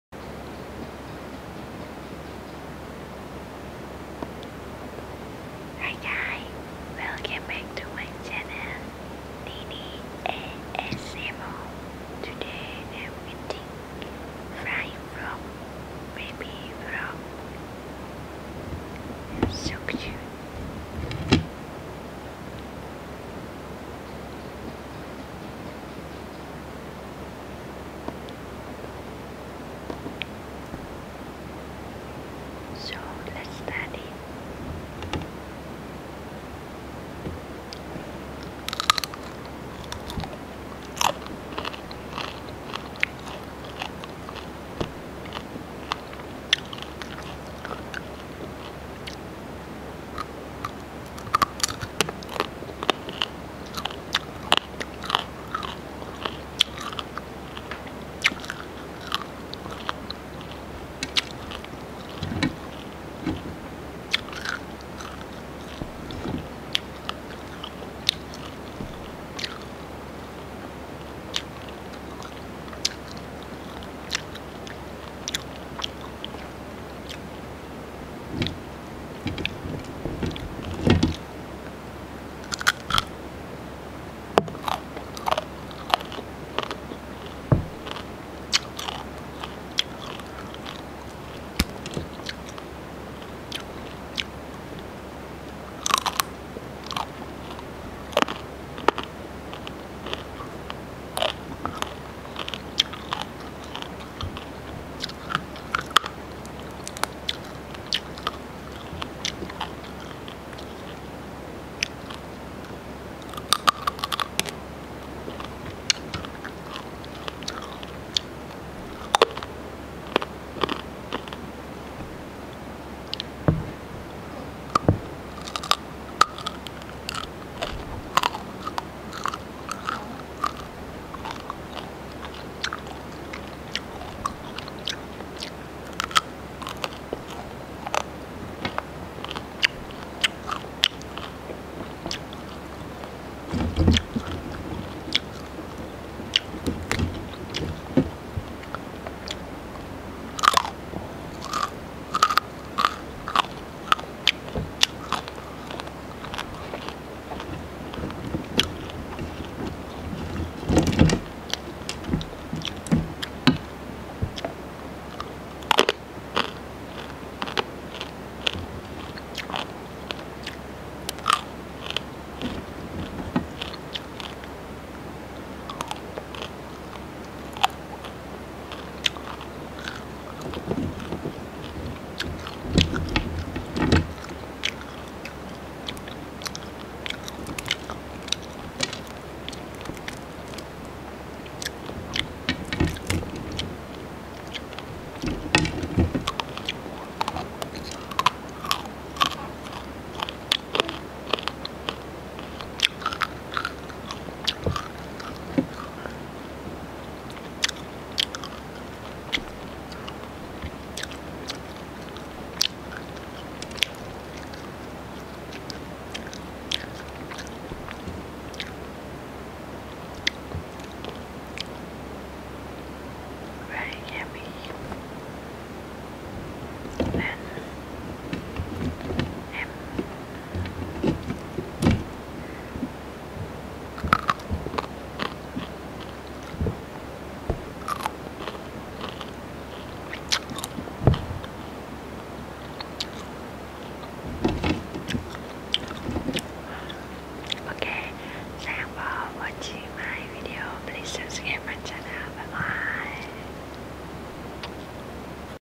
ASMR Deep Fried Frogs(EXTREME CRUNCH EATING SOUNDS)-NYNY ASMR
Hi dea !! this is video I want to show -ASMR Deep Fried Frogs(EXTREME CRUNCH EATING SOUNDS)-NYNY ASMR
Hello If  you like this video and want to get more  with me,
Thank you so much for whaching my videos
hello clike my channel videos me.